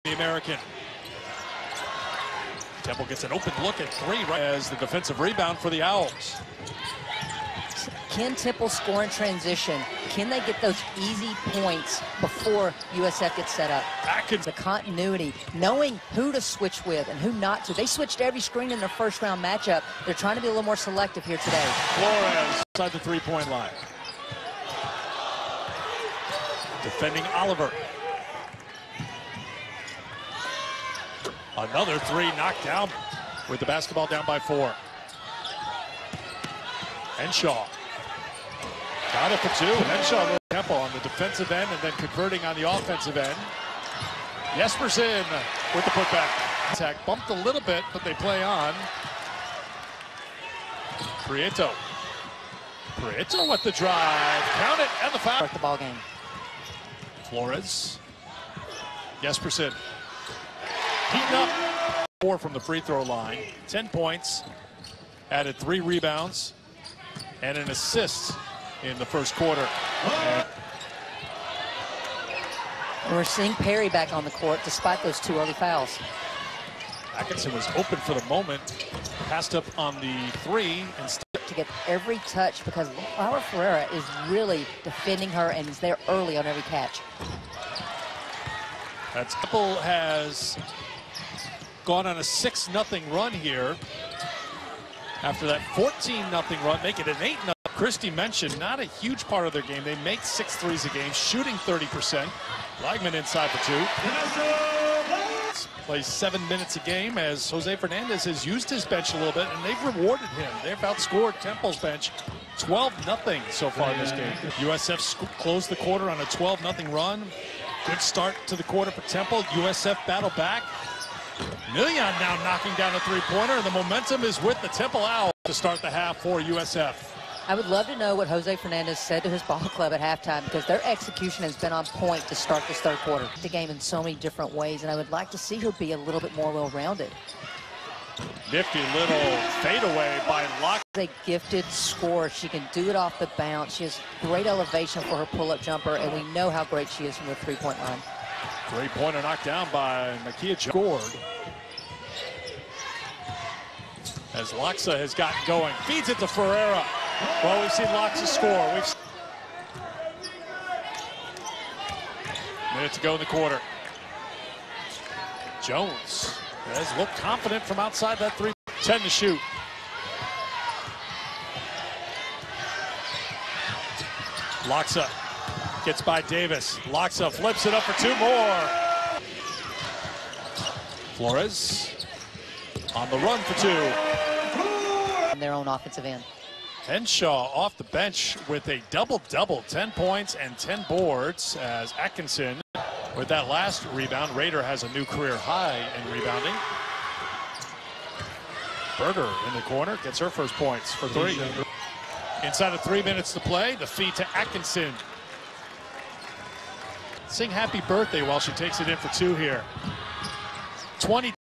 Women's Basketball Highlights - RV/23 USF 76, Temple 60
USF (16-5, 6-2 The American) used a dominant third quarter to secure a 76-60 win over Temple (9-11, 1-6 The American) Sunday afternoon. Four Bulls finished with double-digit numbers in the contest. Maria Jespersen led all scorers with 24 points. Kitija Laksa had 13 points on the night, all of which came in the second half of the game. Laia Flores and Tamara Henshaw rounded out the Bulls' double-figure scorers with 11 and 10, respectively. USF held an advantage in offensive (19-6) and defensive rebounds (35-23).

A trio of Temple players finished with double figures. Tanaya Atkinson had a team-best 22 points, while Mykia Jones and Mia Davis totaled 11 and 10 points, respectively.